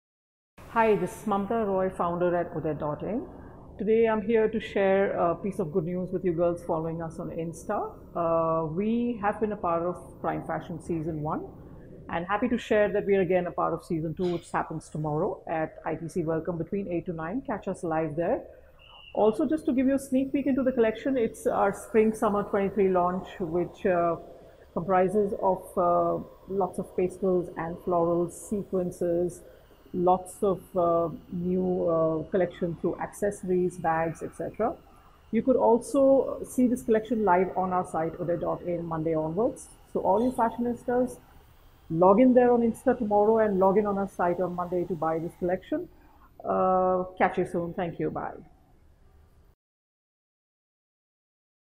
Odette Founder's Desk - Intro Prime Fashion Week Season 2
From The Founder's Desk!!

Catch us going live tomorrow on Prime Fashion Week- Season 2
Between 8 – 9 pm tomorrow, the 17th March 2023

The collection will be live on the website – Odette.in on 20th March 23